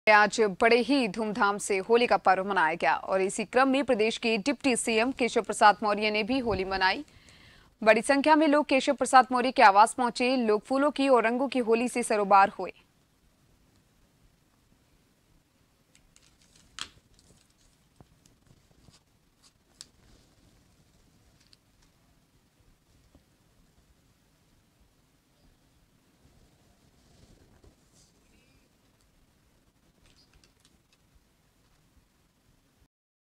Lucknow UP| यूपी में धूमधाम से मनाया गया होली का पर्व,डिप्टी सीएम केशव प्रसाद मौर्य ने भी खेली होली
News Credit - Sk. Banshiwal